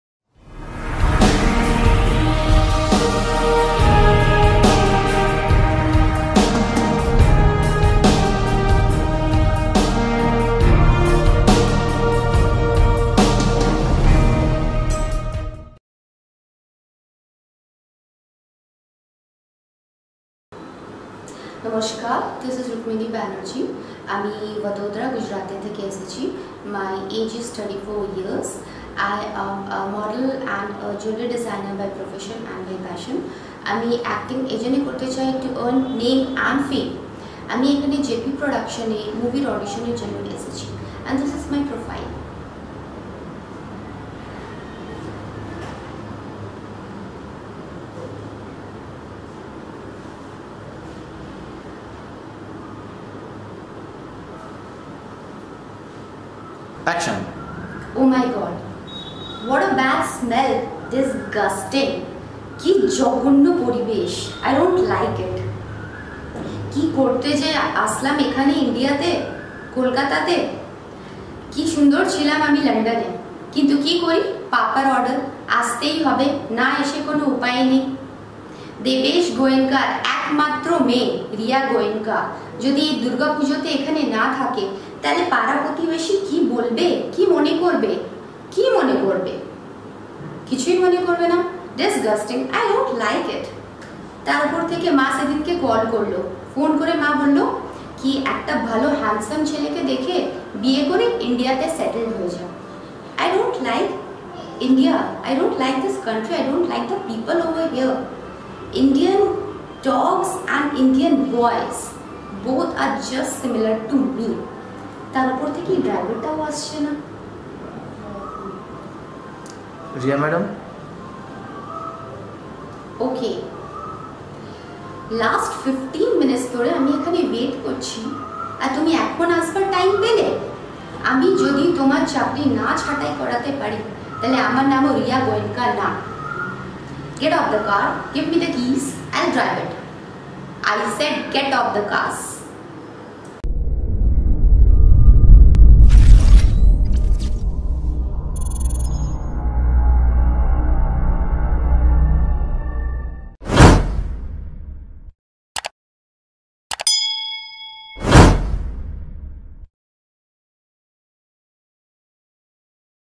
ACTING AUDITION FOR FEMALE ARTISTS FOR UPCOMING MOVIE
All girls & BOYS are Welcome. We are seeking new face (male, female & child artists) For Complete Movie Cast Main lead to Side Roles.

If you are TALENTED, If you’re DEDICATED in Work, If You’re HARD WORKER, If Your ELIGIBLE Then We Give You 100 % Chance.

If Your really Interested & Talented Then Its Right & Wonderful Platform For you To Prove Your TALENT. AUDITION CLOSES SOON. DONT MISS IT !! ALL THE BEST !!!